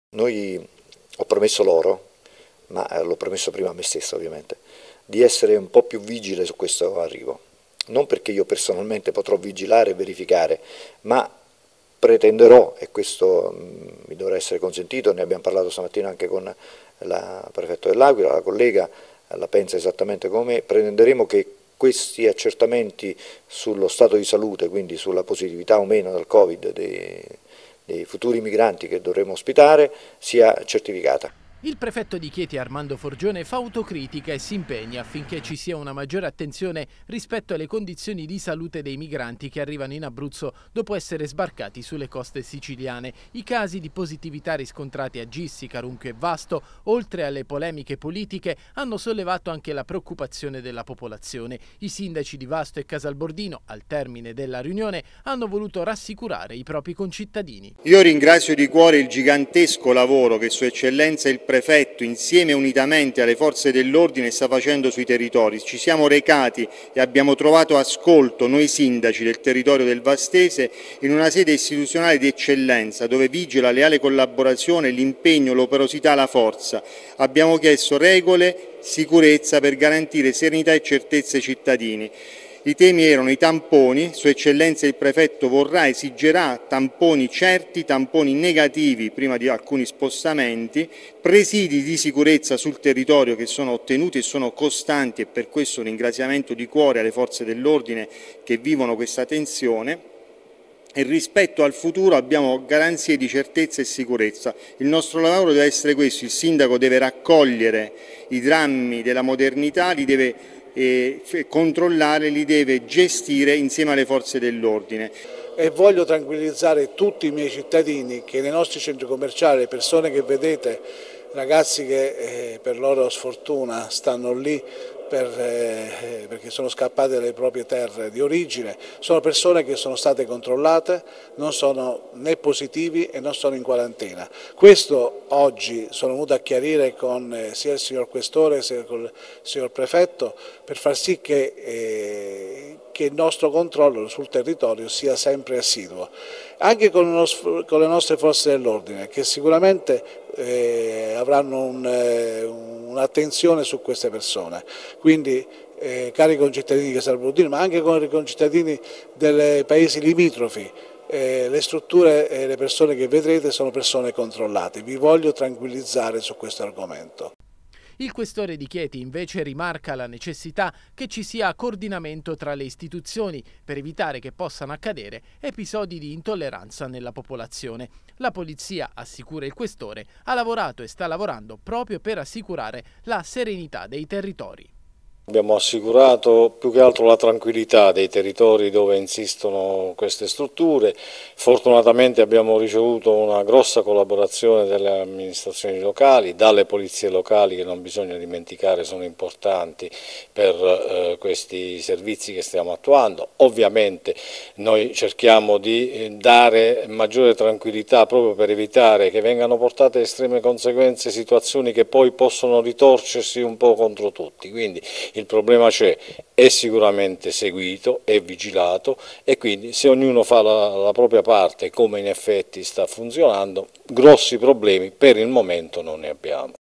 Migranti nel vastese, tavolo tecnico in Prefettura
Questa mattina, a Chieti, nella sede della Prefettura, si è svolta la riunione tra sindaci del vastese Questore e Prefetto per coordinare gli interventi di tutela della popolazione a seguito dei casi di positività al Covid-19 di alcuni migranti nelle strutture di Vasto, Gissi e Carunchio. Il Prefetto ha assicurato che ci sarà maggiore vigilanza e certificazione delle condizioni di salute di chi arriva in Abruzzo